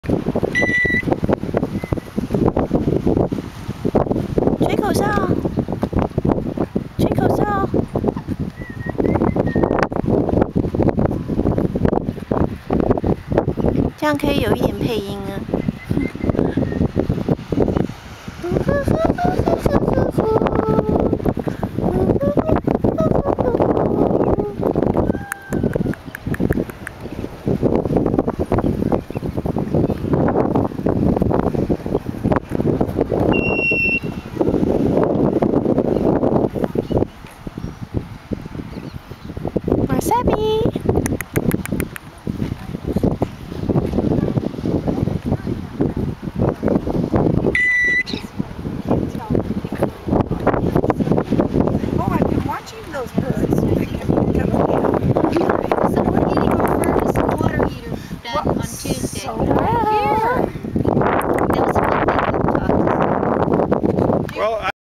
Chewey & Wasabi walking with daddy in Tiburon
caique